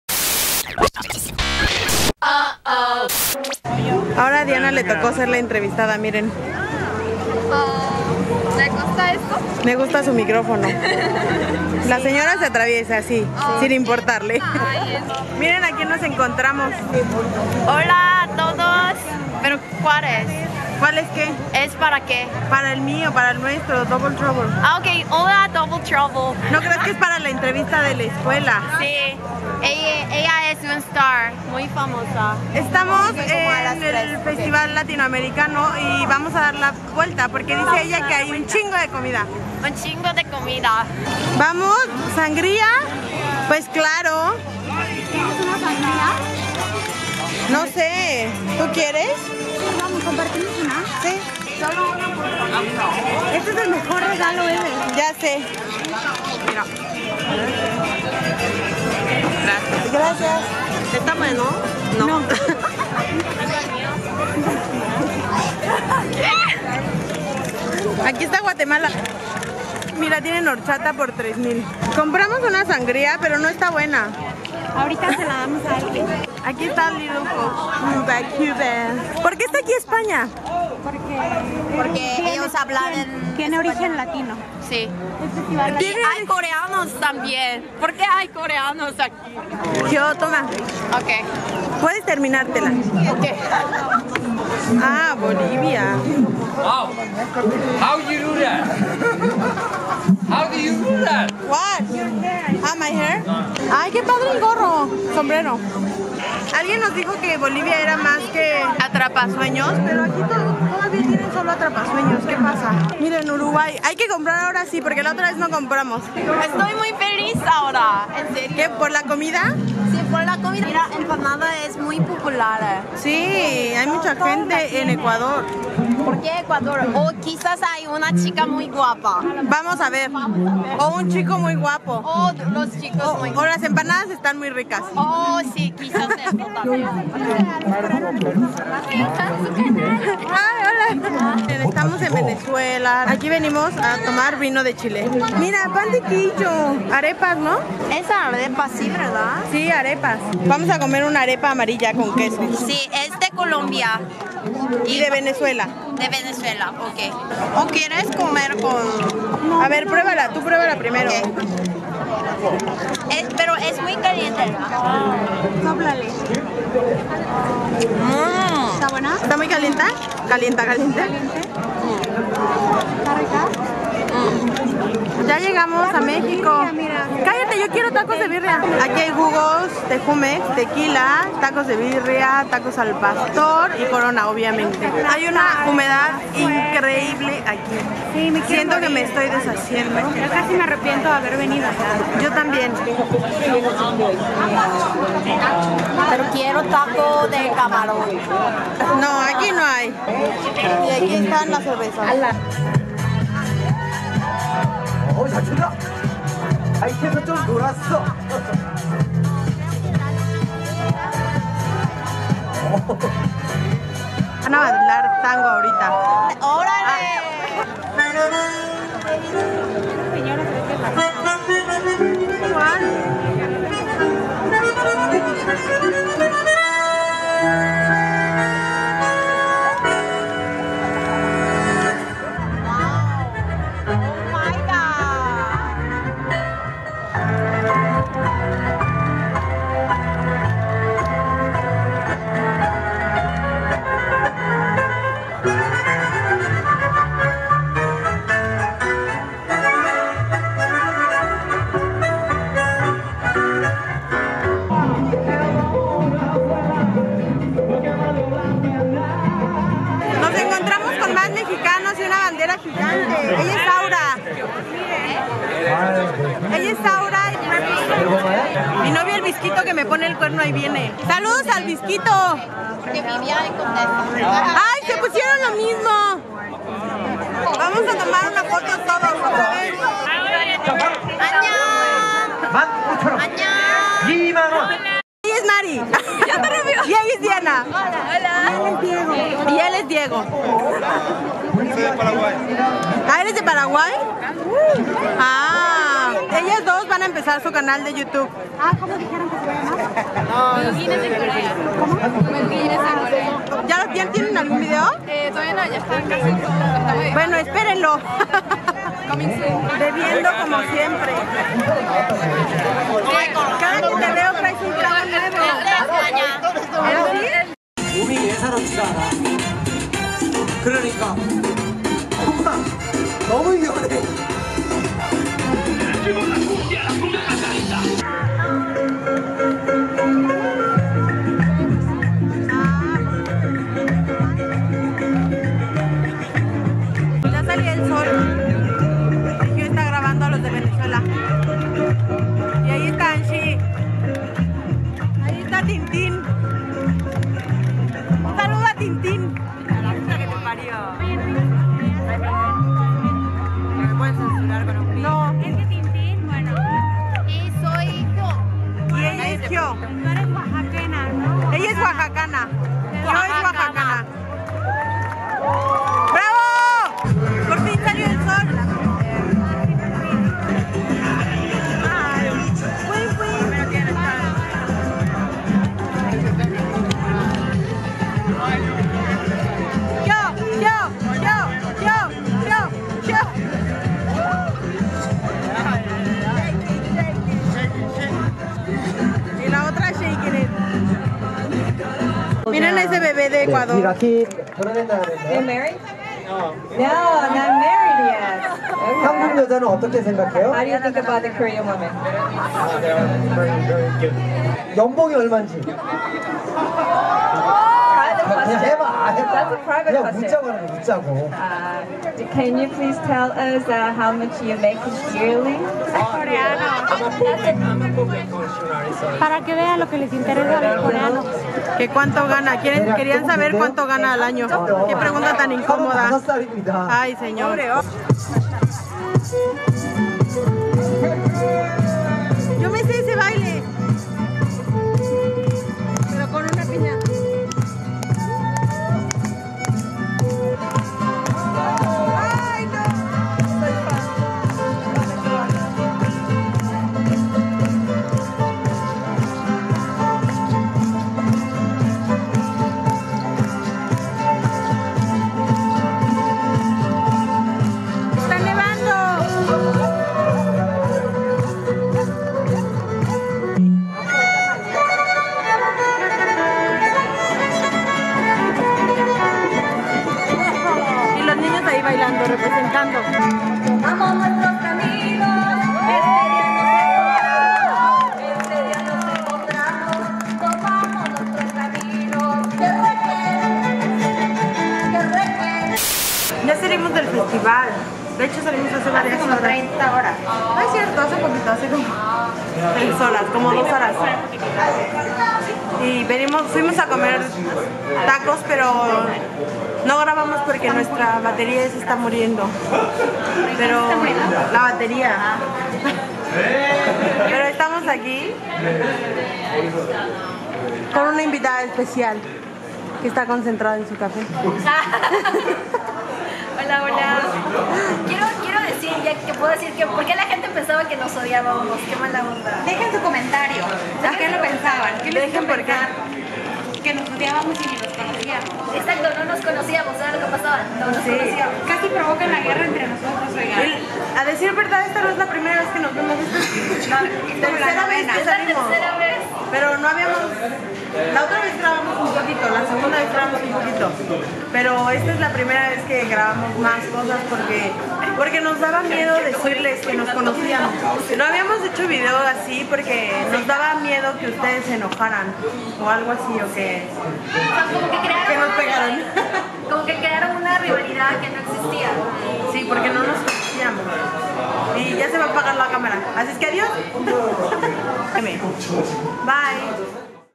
Double Trouble TV 253: Latinos en Corea [Vlogs de la Vida Diaria en Corea] ♥
En el vlog de hoy les compartimos un poco de lo que pasó en el Festival Latinoamericano en Corea, lo pasamos muy bien y esperamos disfruten el video :)

Gisela ⇥ giselaverdinm
Diana ⇥ dverdinm